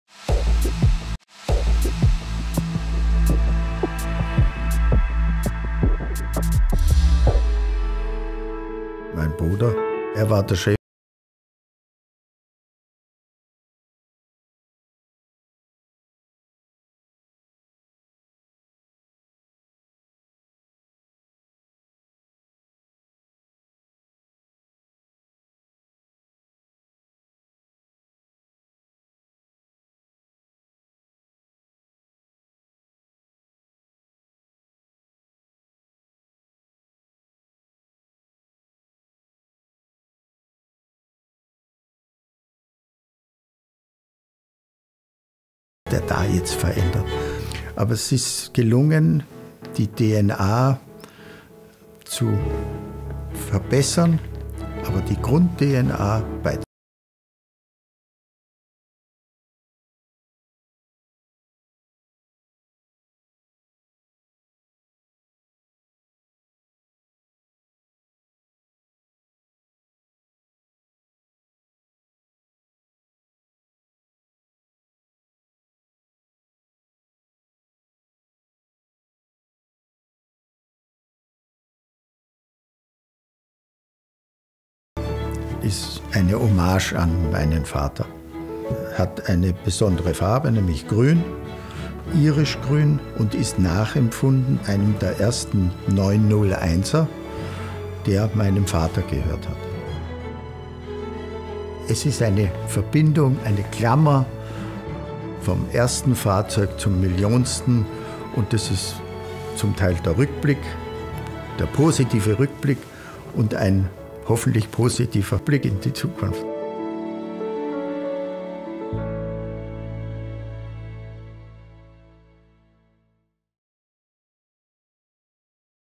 Wolfgang Porsche Interview on the One-millionth 911
Dr. Wolfgang Porsche talks about the one-millionth Porsche 911, a car that is of special significance to him personally, seeing as he was only 20 when his brother Ferdinand designed the original 911.